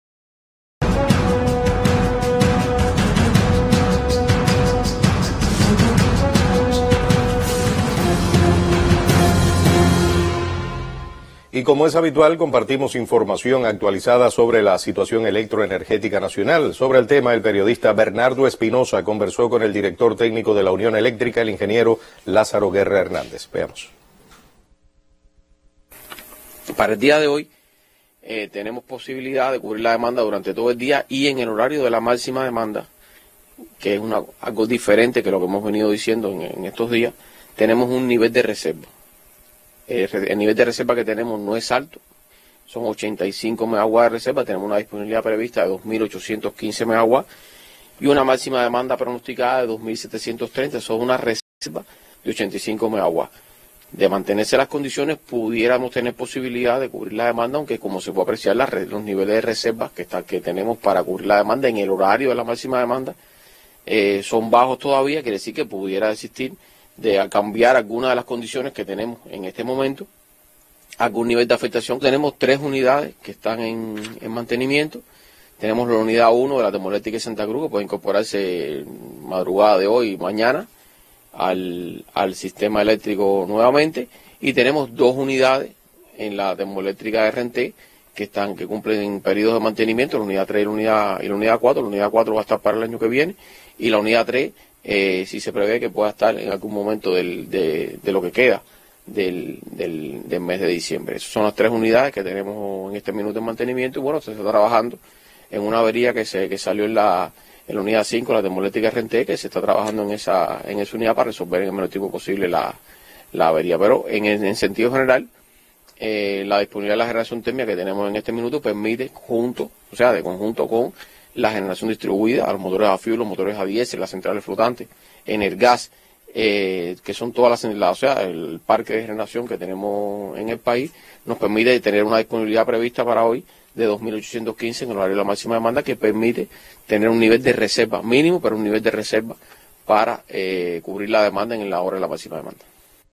¿Cómo se comporta la situación electroenergética en Cuba?
Como es habitual compartimos información actualizada sobre la Situación Electroenergética Nacional (SEN). Sobre el tema el periodista Bernardo Espinosa conversó con el Director Técnico de la UniónEléctrica, (UE) de Cuba, el ingeniero Lázaro Guerra Hernández.